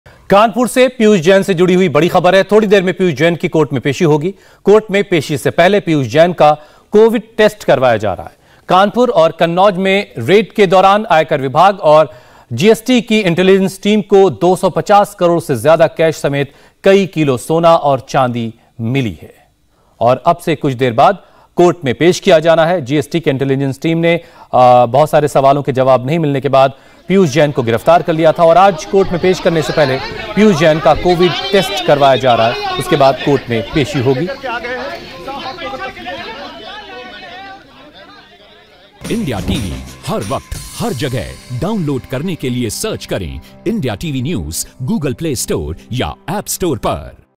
कोर्ट में पेश करने से पहले Piyush Jain का करवाया गया Corona Test
कोर्ट में पेश करने से पहले Piyush Jain का कोरोना टेस्ट करवाया गया है। पीयूष जैन के पास 257 करोड़ का कैश अब तक मिला है।